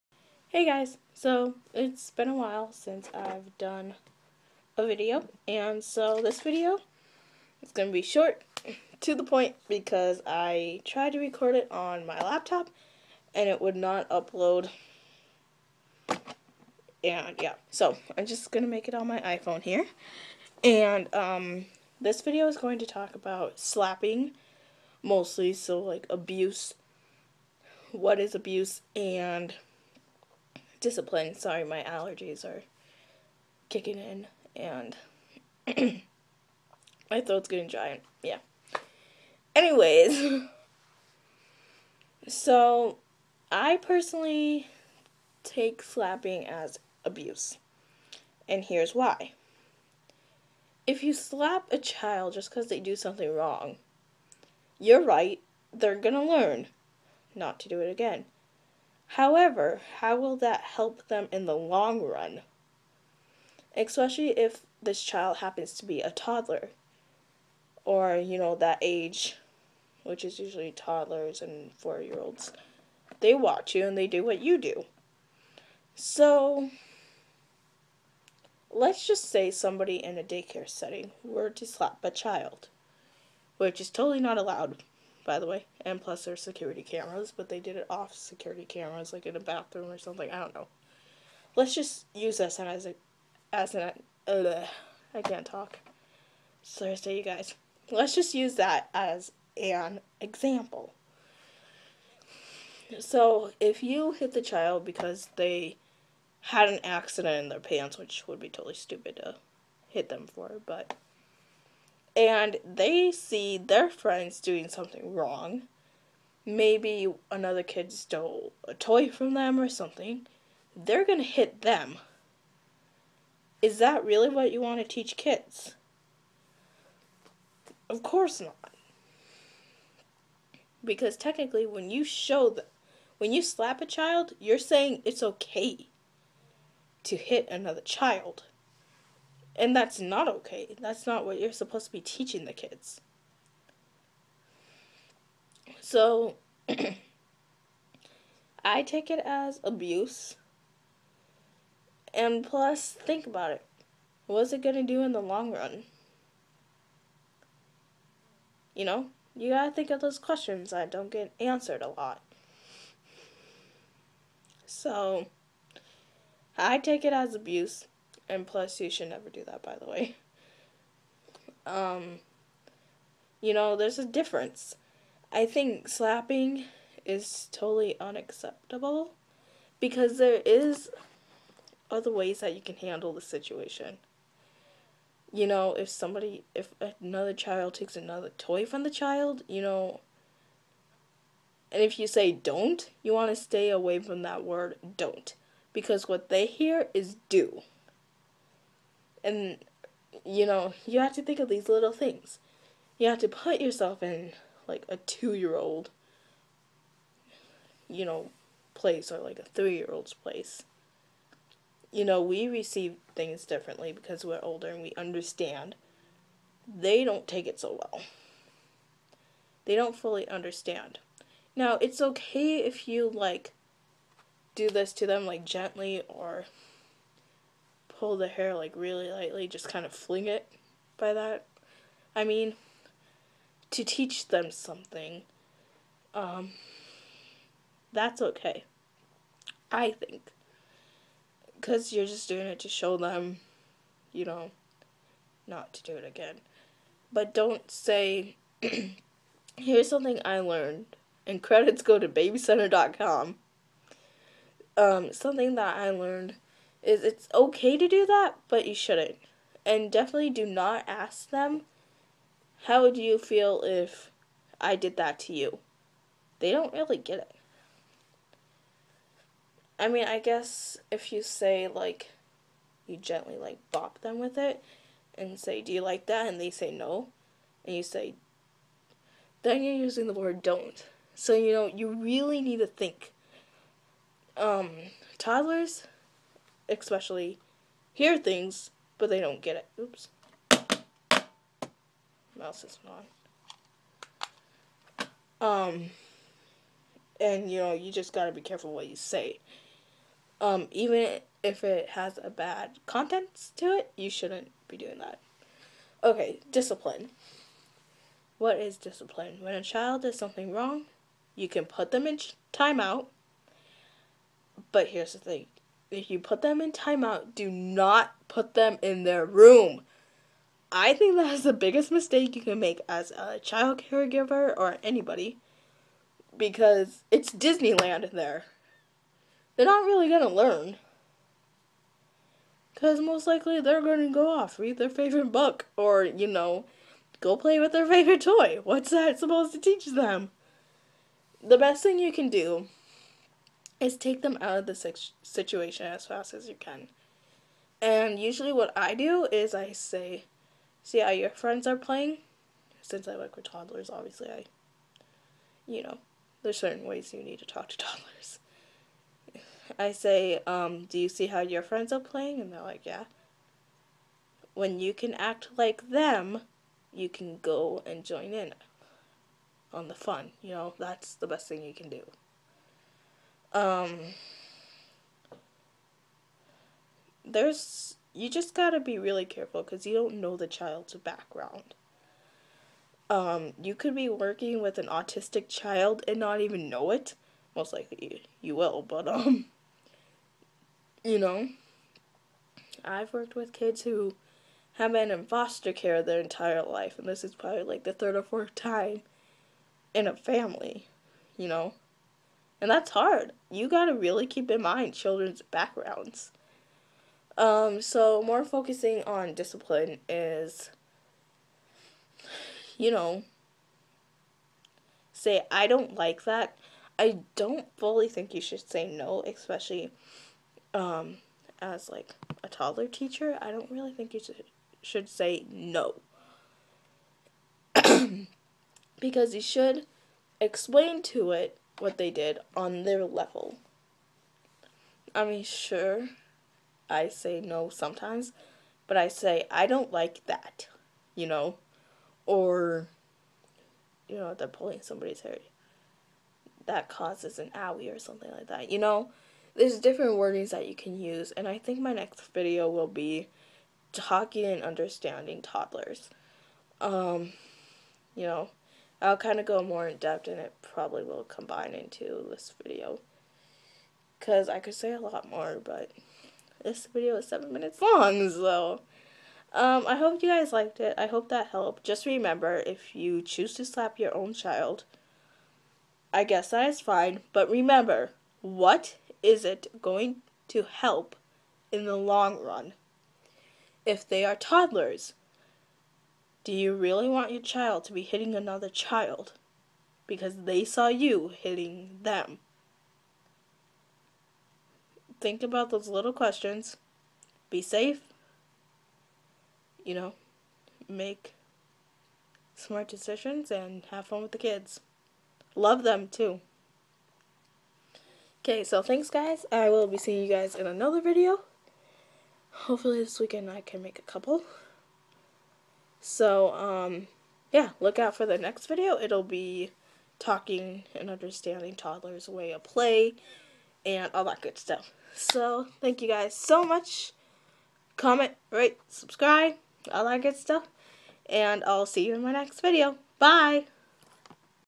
Abuse vs Discipline
Here are a few tips for discipline. This video explains slapping and if it is abuse or not. Watch and check it out